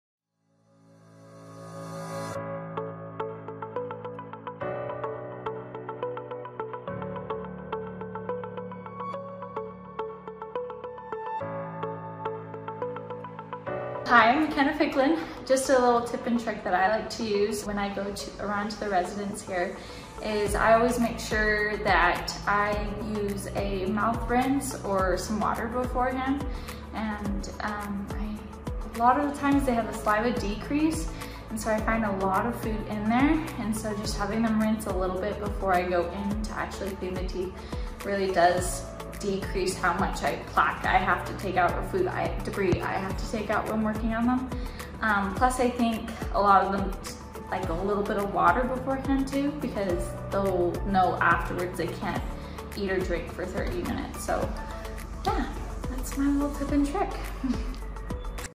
Short Clips and Tips: Decreasing Debris and Improving Comfort Before Assisted Oral Hygiene
Ms. McKenna Fickland (our student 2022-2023) now RDH shares with you a quick tip to make assisted oral hygiene easier on the provider and more comfortable for the resident. A simple rinse can make a big difference. One caution with rinsing is make sure they are allowed water without thickener and that they are capable of the swish and spit. If they can't swish and spit, then a wipe with a wet 2x2 or mouth swab can help as well.